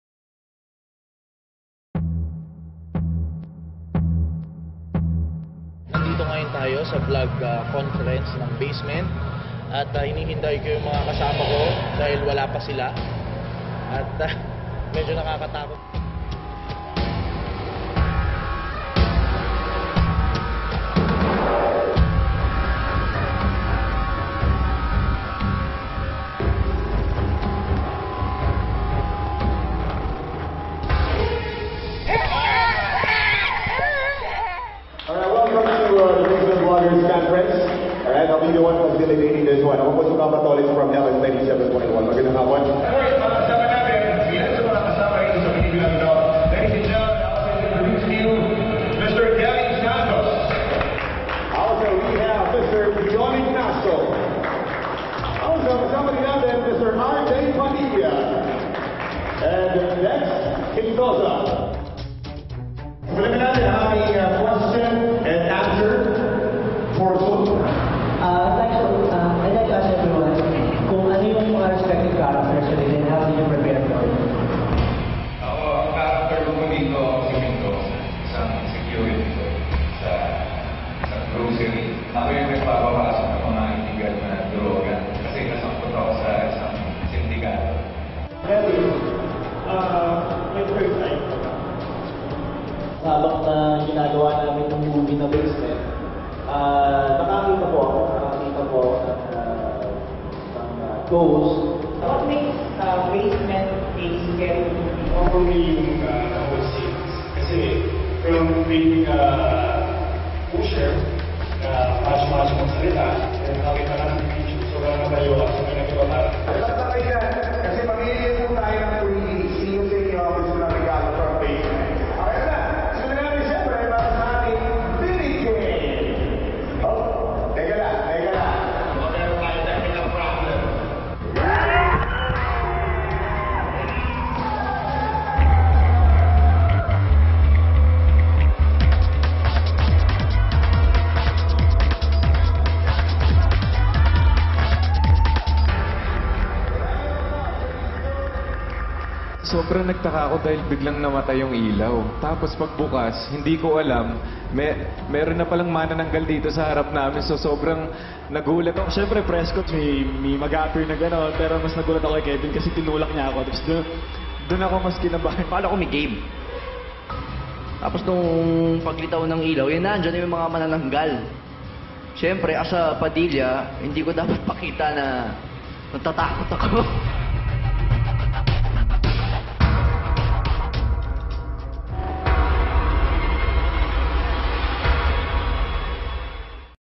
"Ano'ng ikinagulat ng 'Basement' boys na sina Dion, Dex, Kevin at RJ?"
Akala ng Basement boys na sina Dion Ignacio, Dex Quindoza, Kevin Santos at RJ Padilla ay  isang bloggers' conference lamang ang kanilang dadaluhan. Ngunit laking gulat nila nang sinundan pala sila dito ng nilalalang mula sa pelikula. Panoorin: